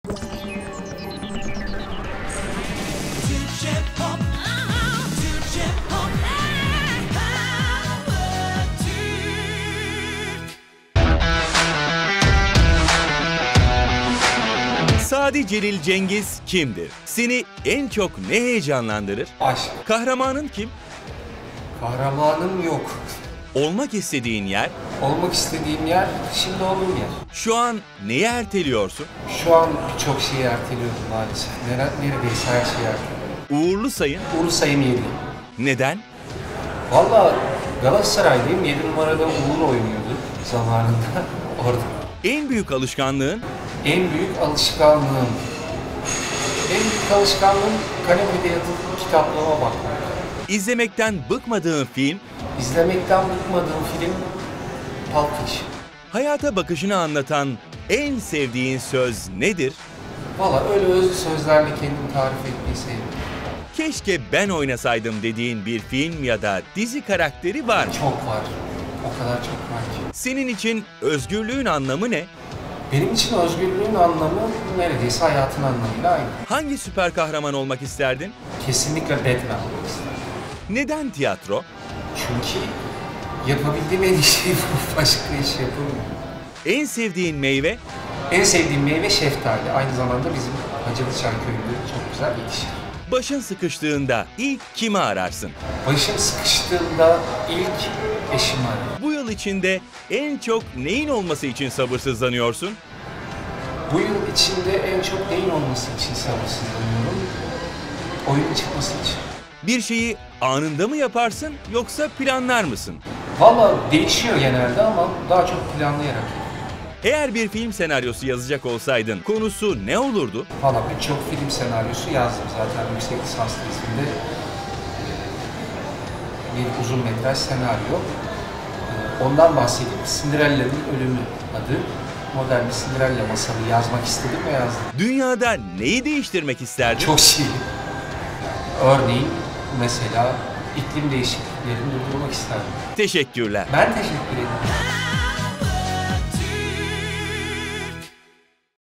Sadi Celil Cengiz Kimdir?
Sevilen oyuncu Sadi Celil Cengiz, hakkında bilinmeyen ve merak edilen soruları cevapladı. Bu eğlenceli röportaj şimdi sizlerle.

Yıldız şarkıcılar, oyuncular ve yıldızı parlayan yepyeni isimler PowerTürk'e  özel cevaplarıyla Kimdir'de.